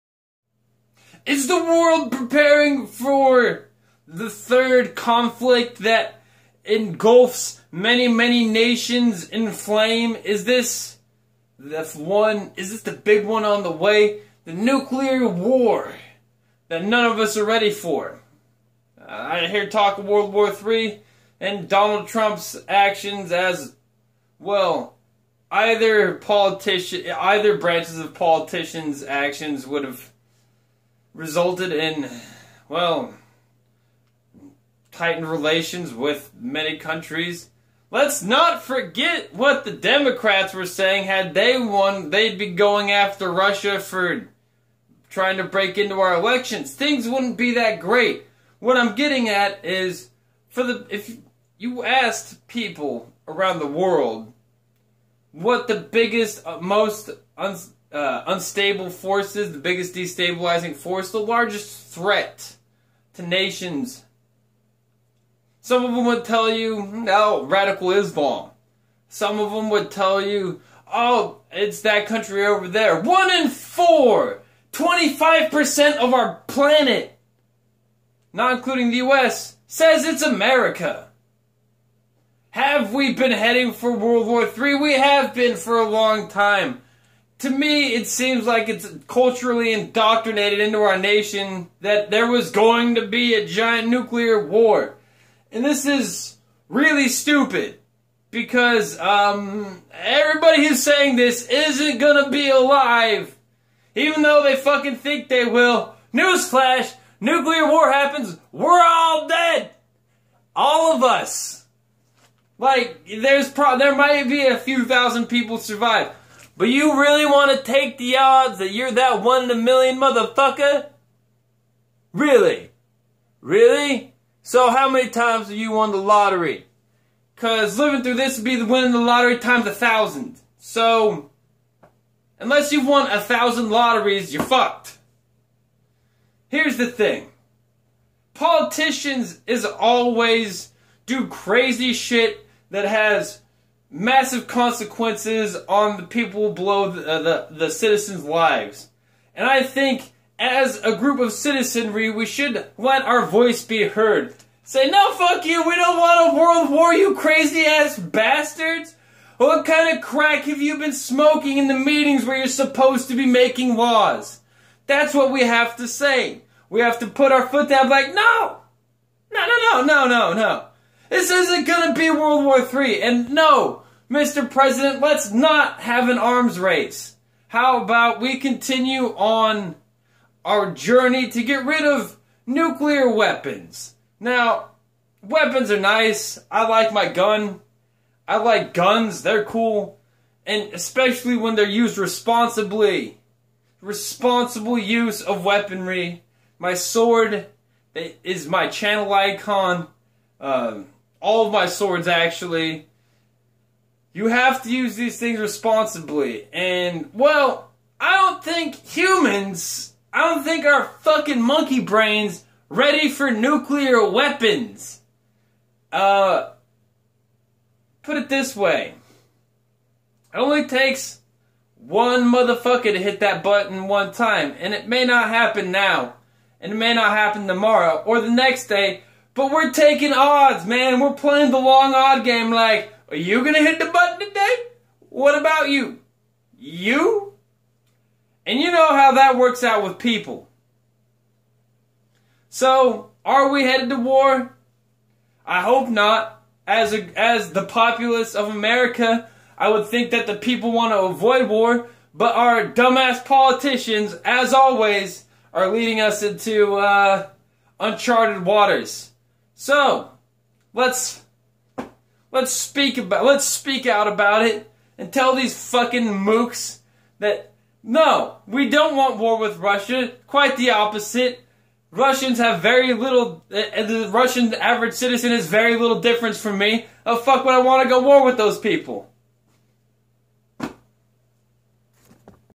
Really talk of world war 3 really?
Look only the dumdums in power want war because they seek more of what they already have.